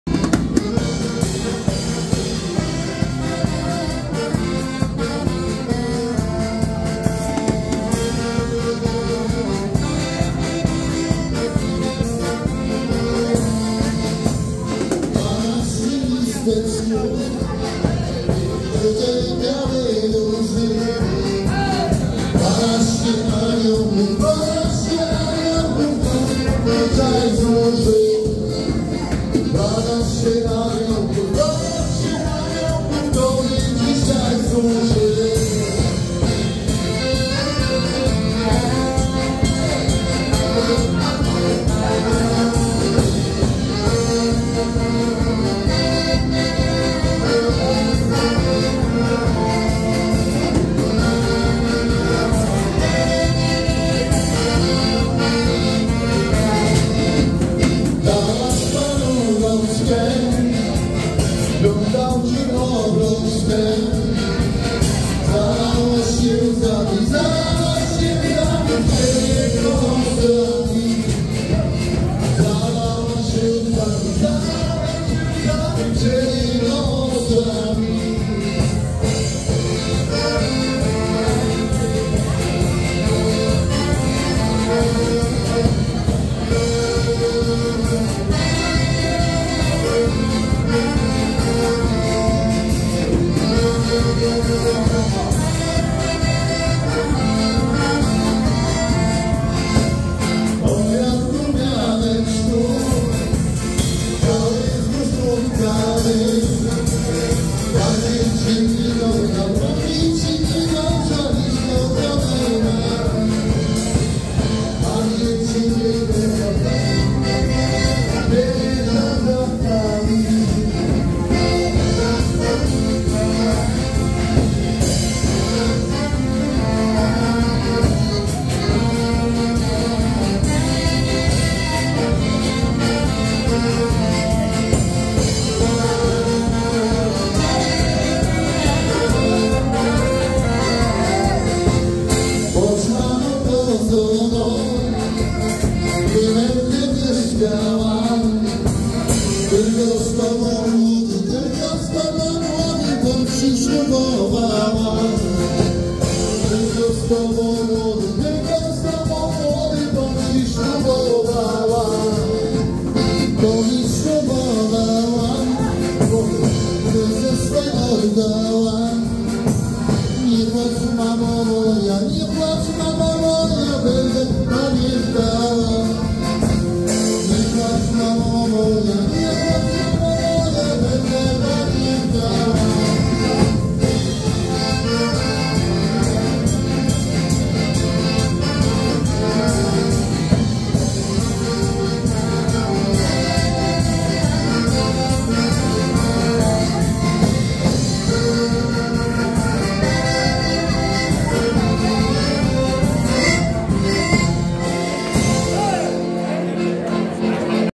Świetne tancerki pokazały swoje umiejętności taneczne.
Zabawa ludowa w Wygnanowie z kapelą Tomasza Stachury. ZAPRASZAM DO SUBSKRYPCJI NASZEGO KANAŁU.